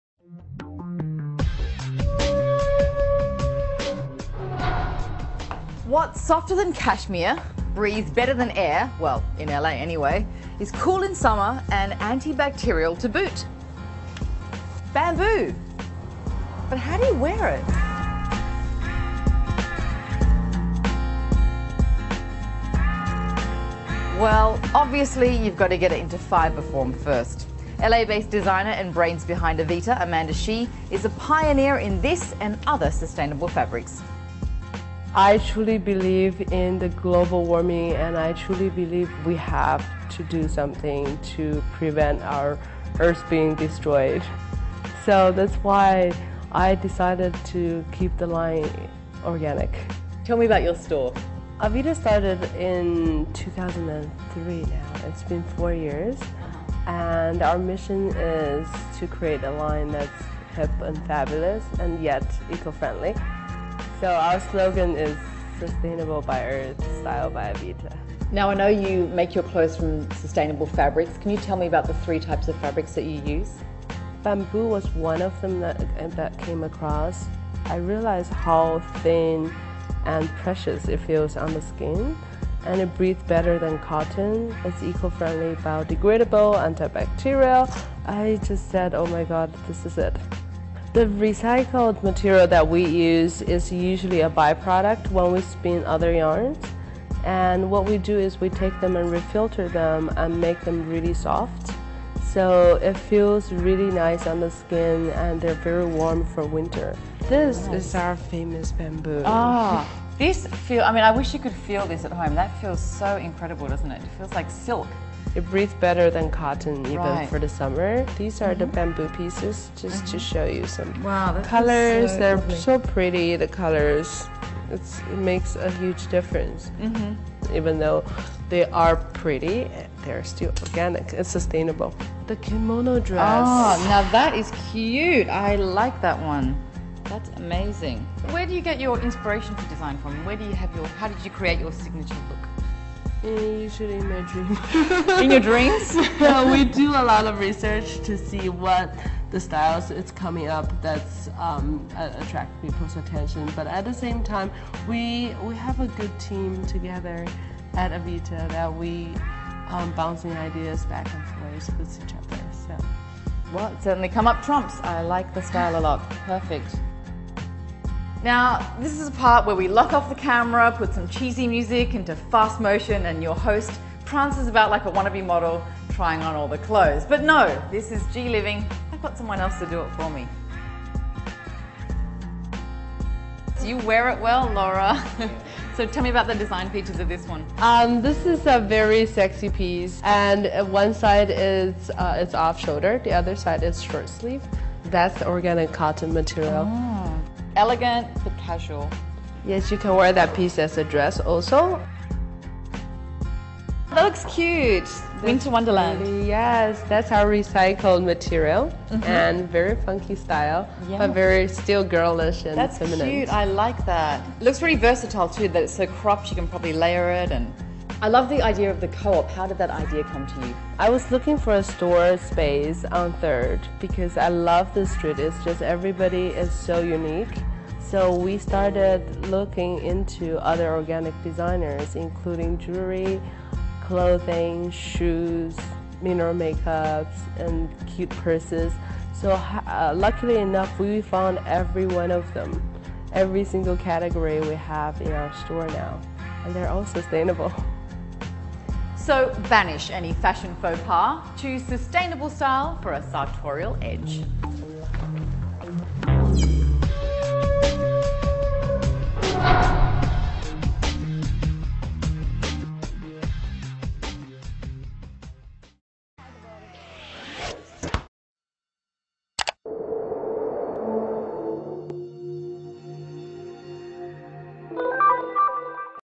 Avita Bamboo Fashion | G LIVING LIVE
Join Sarah Backhouse on G LIVING LIVE, as she explores the new trend of Bamboo fashion.